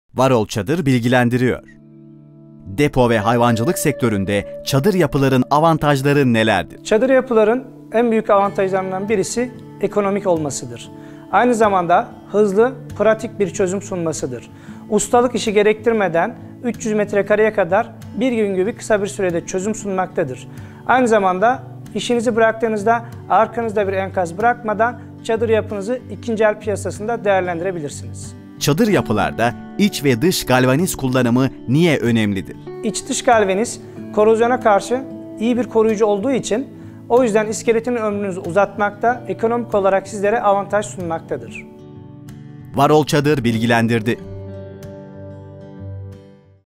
"Varol Çadır Bilgilendiriyor" Depo ve Hayvancılık Sektöründe Çadır Yapıların Avantajları Nelerdir?
"Varol Çadır Bilgilendiriyor"
Depo ve Hayvancılık Sektöründe Çadır Yapıların Avantajları Nelerdir?
Çadır Yapılarda İç ve Dış Galvaniz Kullanımı Niye Önemlidir?